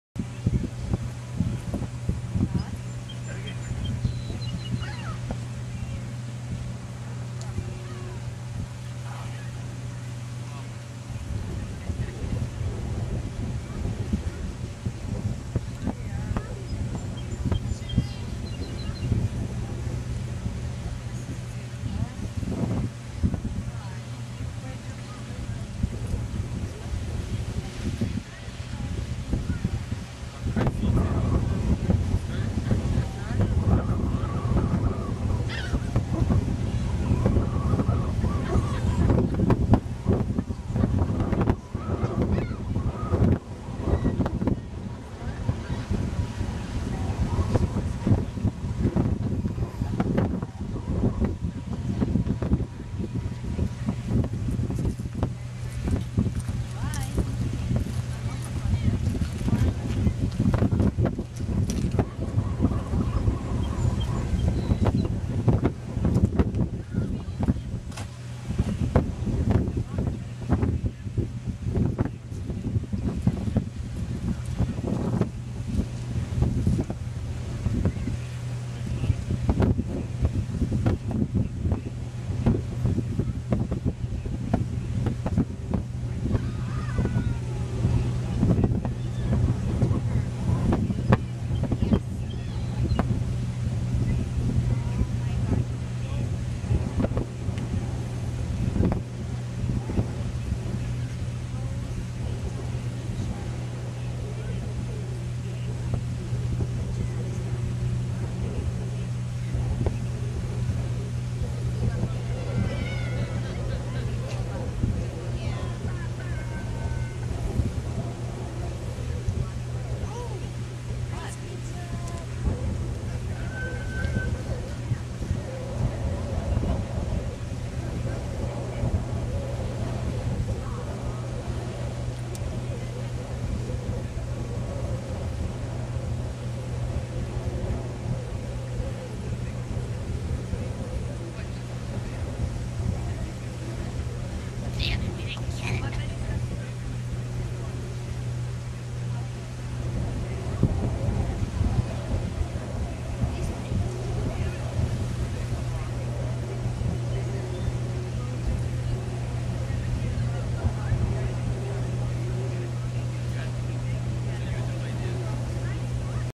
The Proposal
I proposed to my girlfriend on July 18, 2009 at Fort Phoenix Beach in Fairhaven, MA.  She said YES!